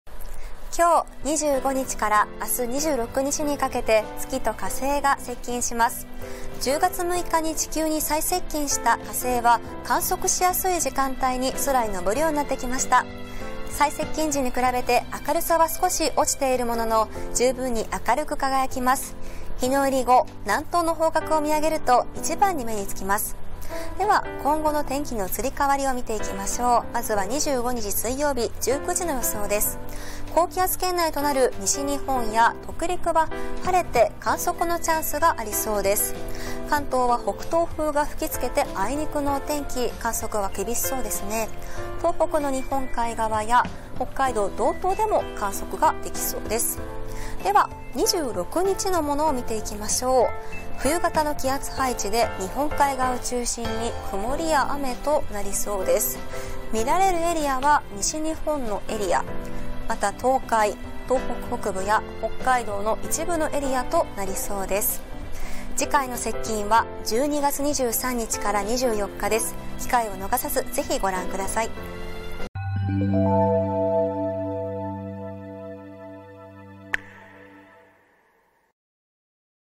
25日(水)〜26日(木) 月と火星が接近
今日25日(水)から明日26日(木)にかけて、月と火星の接近が見られます。
夜は空を見上げてみてはいかがでしょうか。